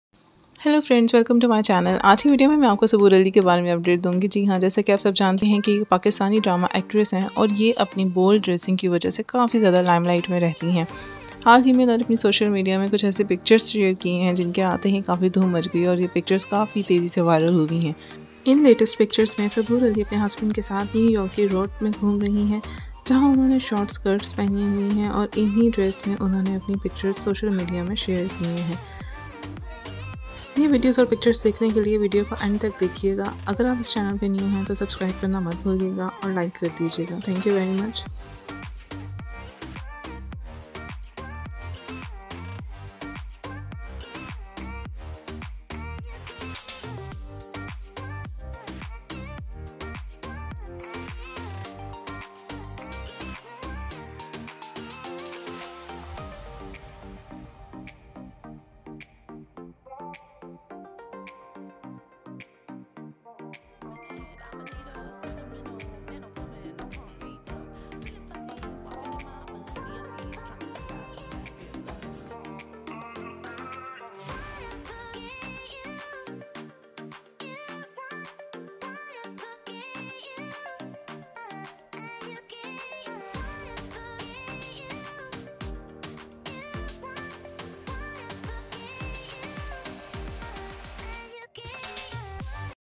Bold dressing and pictures of Saboor Aly from new York Brooklyn bridge
hello

In this video you will see the famous actress saboor aly with her husband roaming in the roads of New York City.